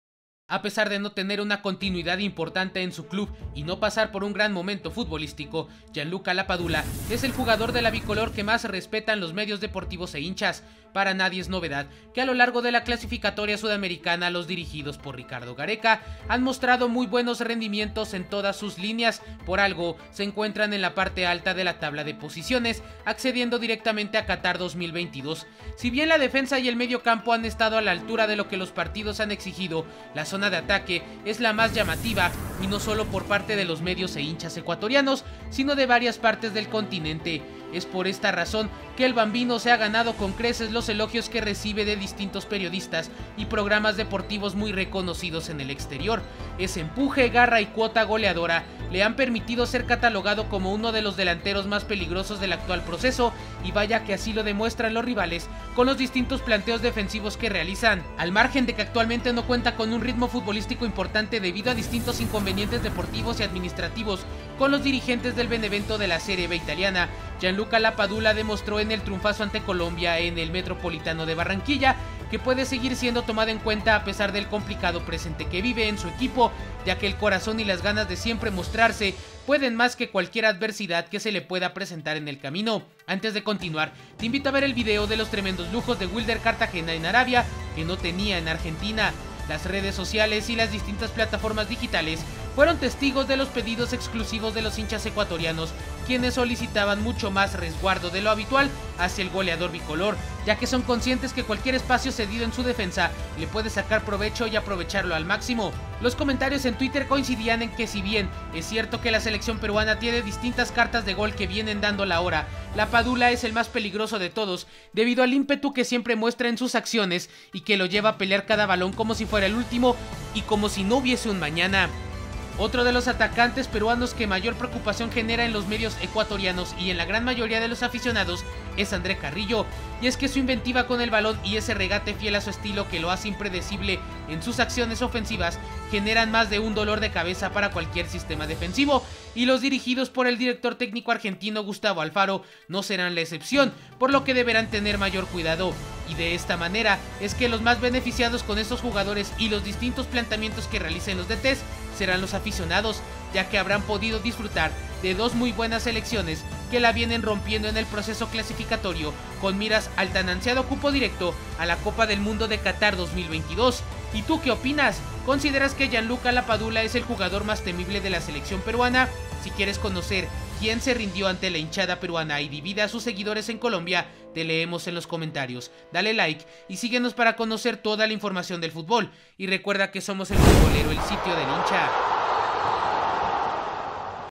🔥 El jugador de la BICOLOR 🤔 que más respetan los medios deportivos y los hinchas. 😎⚽
GianlucaLapadula es el jugador de la blanquirroja que mayor respeto y resguardo genera en los medios e hinchas ecuatorianos.